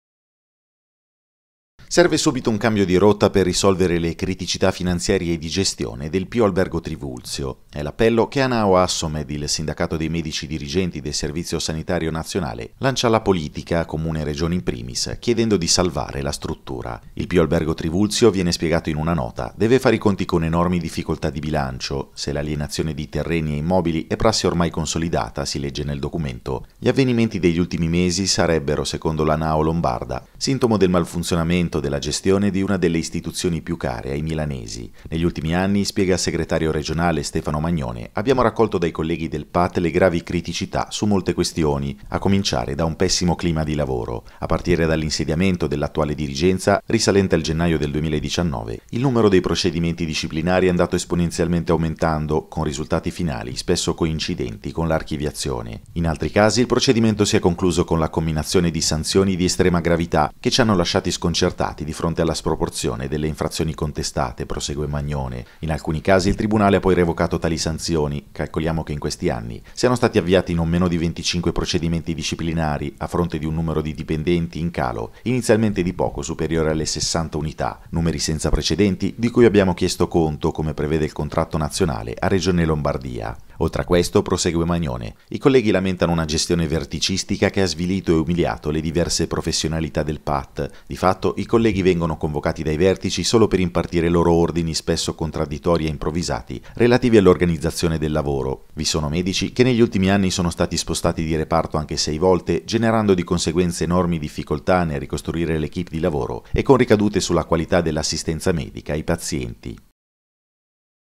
Milano, l'appello di Anaao-Assomed: ”Salvate il Pio Albergo Trivulzio”
Serve subito un cambio di rotta per risolvere le criticità finanziarie e di gestione del Pio Albergo Trivulzio. E' l'appello che Anaao-Assomed, il sindacato dei medici dirigenti del Servizio sanitario nazionale, lancia alla politica, Comune e Regione in primis, chiedendo di "salvare" la struttura. Il Pio Albergo Trivulzio, viene spiegato in una nota, deve fare i conti con enormi difficoltà di bilancio.

Se l'alienazione di terreni e immobili è "prassi ormai consolidata", si legge nel documento, gli avvenimenti degli ultimi mesi sarebbero secondo l'Anaao lombarda "sintomo del malfunzionamento della gestione di una delle istituzioni più care ai milanesi". "Negli ultimi anni - spiega il segretario regionale Stefano Magnone - abbiamo raccolto dai colleghi del Pat le gravi criticità su molte questioni, a cominciare da un pessimo clima di lavoro. A partire dall'insediamento dell'attuale dirigenza, risalente al gennaio 2019, il numero dei procedimenti disciplinari è andato esponenzialmente aumentando, con risultati finali spesso coincidenti con l'archiviazione".

"In altri casi il procedimento si è concluso con la comminazione di sanzioni di estrema gravità, che ci hanno lasciati sconcertati di fronte alla sproporzione delle infrazioni contestate, prosegue Magnone. In alcuni casi il tribunale ha poi revocato tali sanzioni. Calcoliamo che in questi pochi anni siano stati avviati non meno di 25 procedimenti disciplinari, a fronte di un numero di dipendenti, in calo, inizialmente di poco superiore alle 60 unità: numeri senza precedenti, di cui abbiamo chiesto conto, come prevede il contratto nazionale, a Regione Lombardia". "Oltre a questo - conclude Magnone - i colleghi lamentano una gestione verticistica che ha svilito e umiliato le diverse professionalità del Pat. Di fatto i colleghi vengono convocati dai vertici solo per impartire loro ordini, spesso contraddittori e improvvisati, relativi all'organizzazione del lavoro. Vi sono medici che negli ultimi anni sono stati spostati di reparto anche 6 volte, generando di conseguenza enormi difficoltà nel ricostruire le équipe di lavoro e con ricadute sulla qualità dell'assistenza medica ai pazienti".